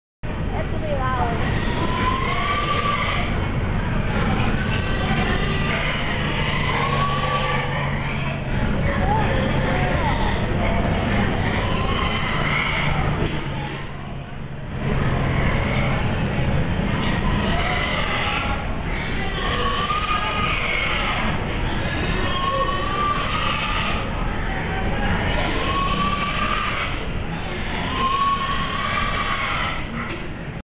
Pigs at the fair
We visited the Winona County fair and checked out the animals. These two pigs were having a little standoff it seemed. One would stick its snout into the other's pen and then they would go back and forth. Guess it's a good way to pass the time...